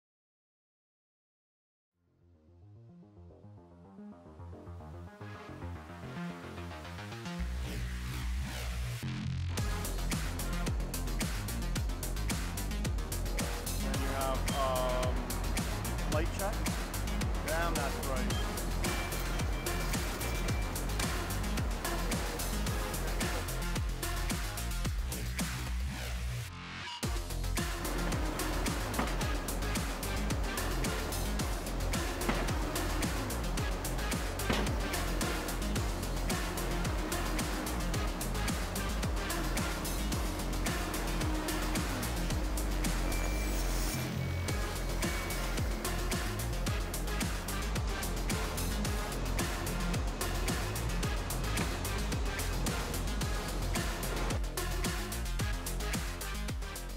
See the Super Bright CR700 Combo Receiver in Action
Whether you're on a machine like an excavator or dozer or working with a grade rod, the CR700 Combo Receiver has you covered.

The CR700 has the ability to mount directly onto a machine or grade rod or seamlessly switch in between. With its intuitive design and Super Bright LEDs, you get instant visual feedback on your elevation.

Stay on Grade with the CR700 Combo Receiver from Spectra Precision.